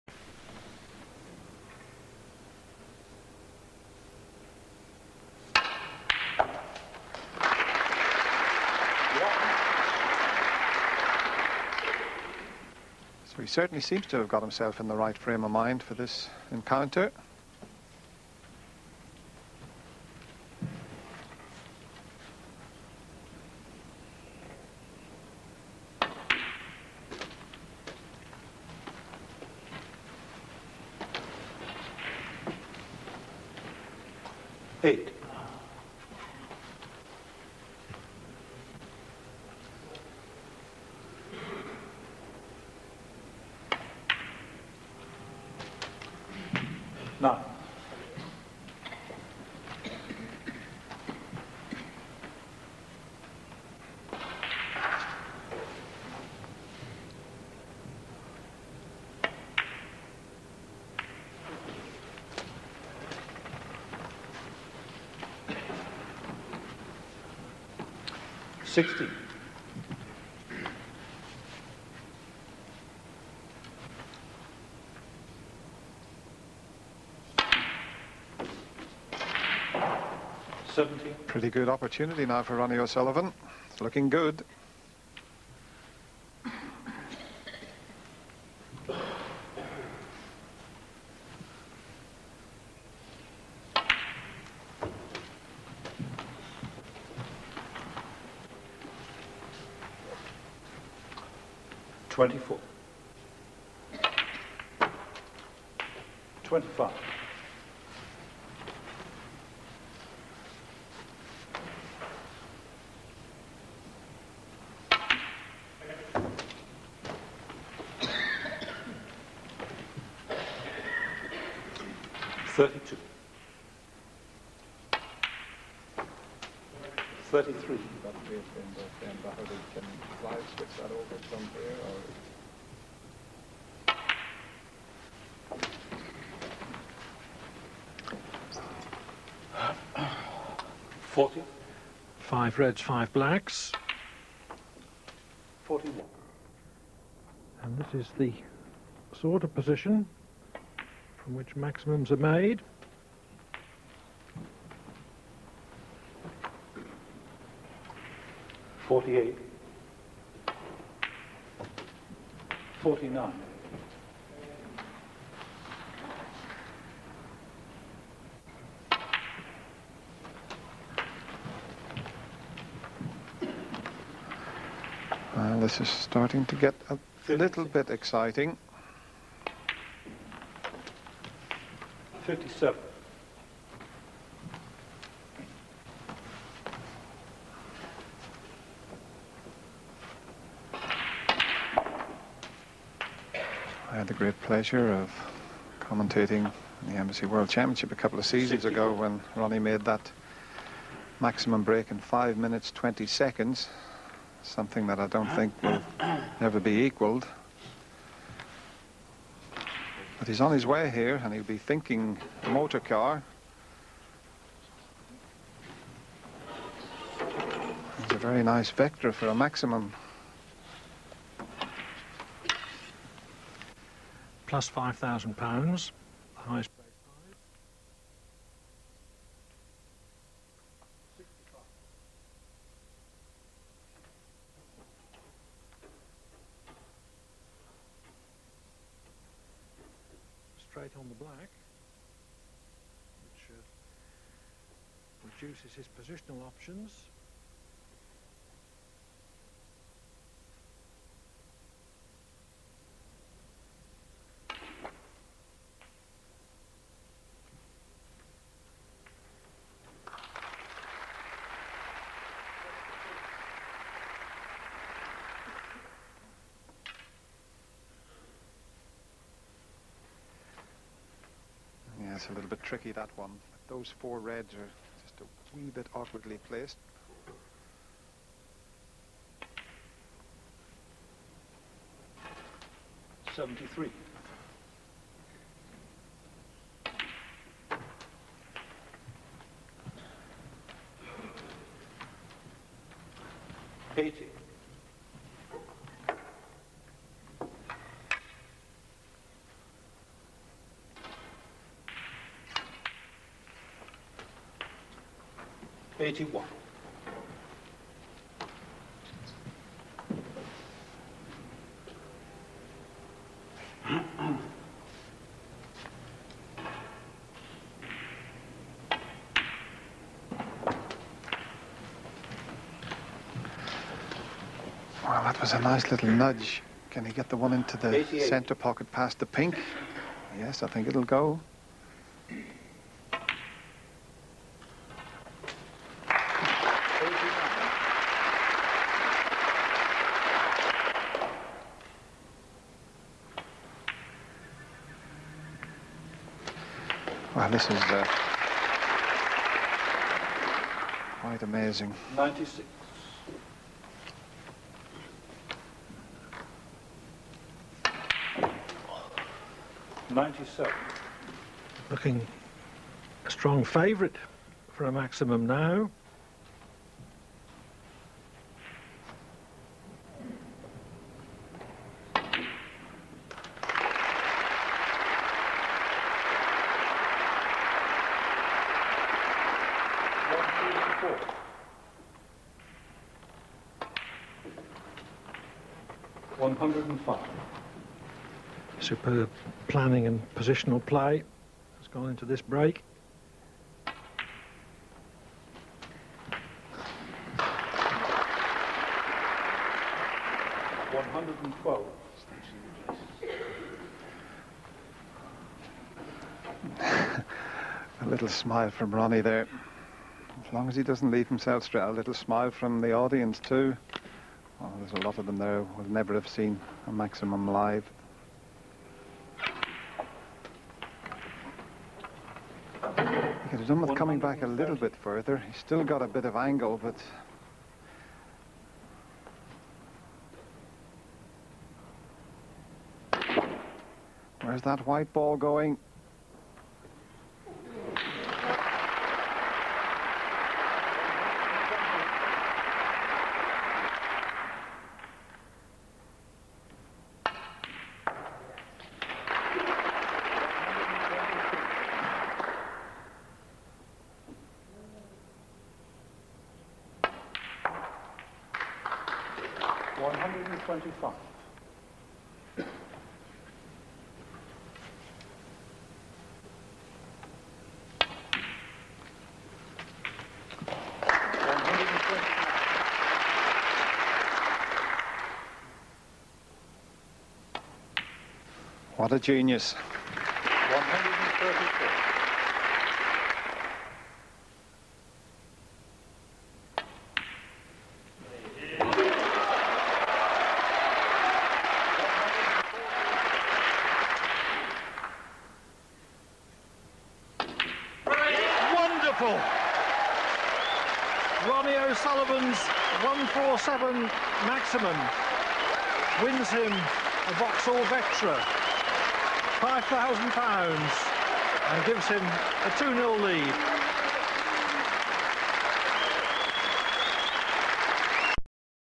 Ronnie The Rocket O'Sullivan vs. Graeme Dott - Grand Prix - 147 breaks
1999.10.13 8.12 Grand Prix (w. Graeme Dott)